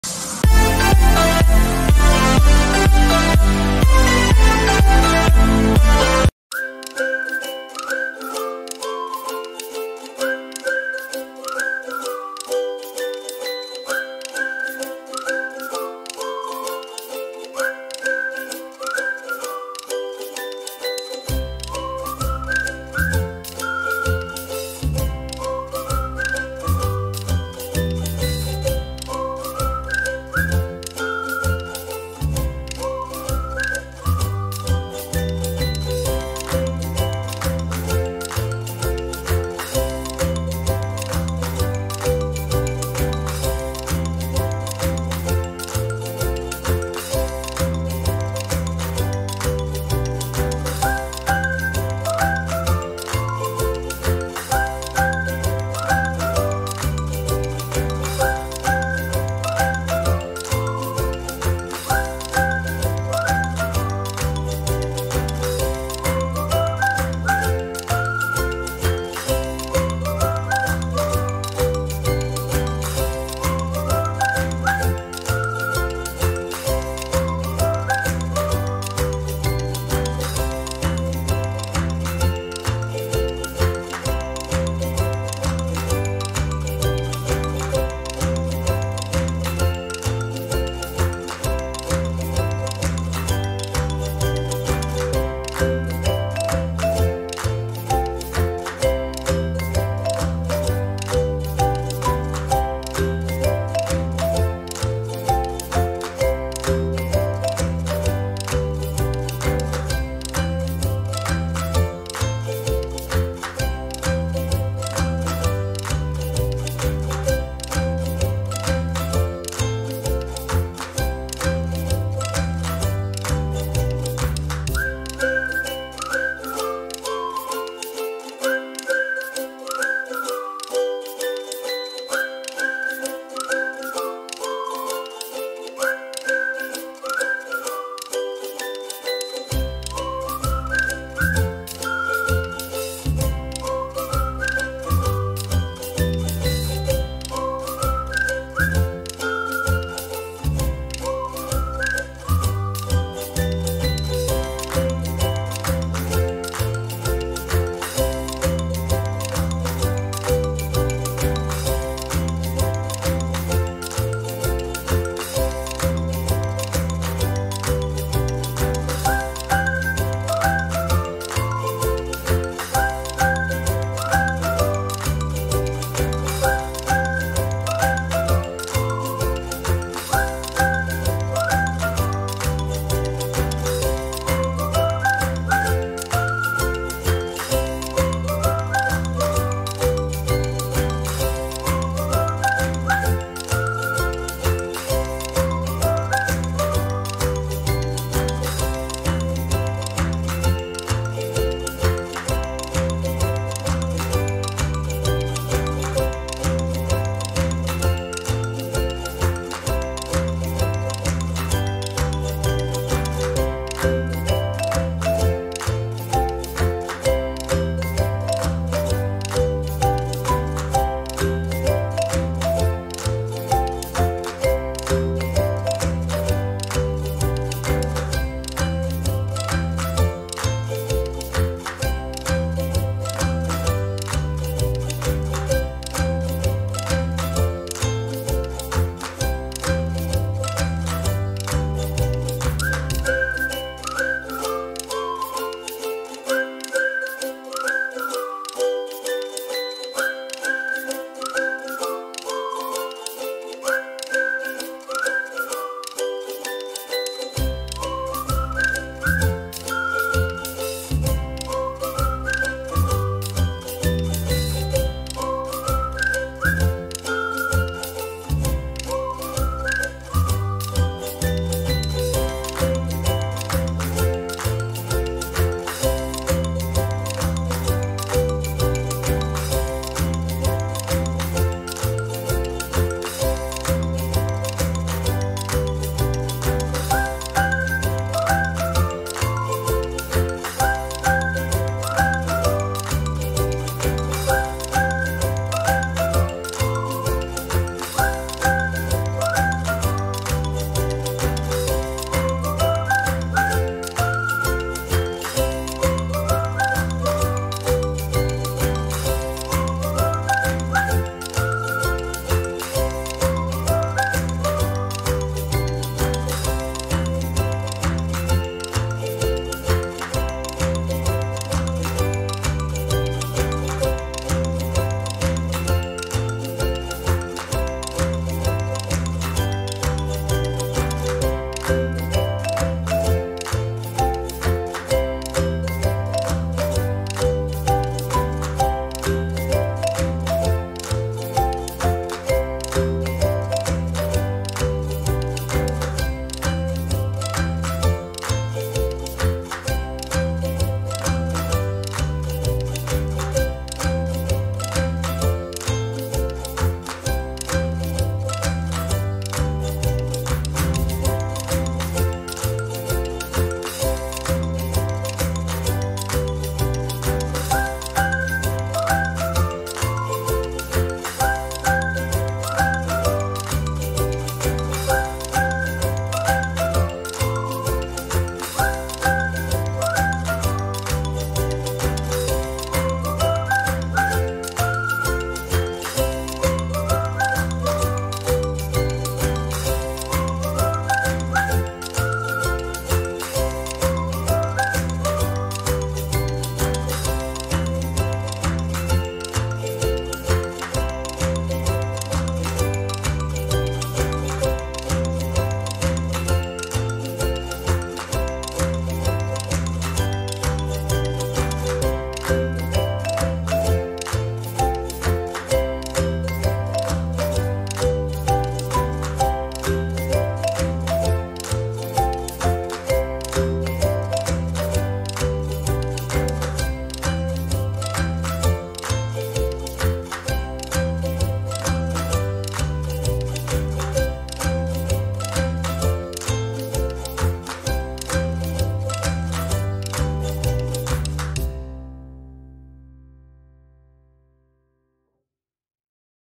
GK AND GENERAL STUDY (OPSC)
There are 20 MCQ in this video , all the aspirant should try to answer each question within 10 sec. (we have set up a 10 sec. timer for that ).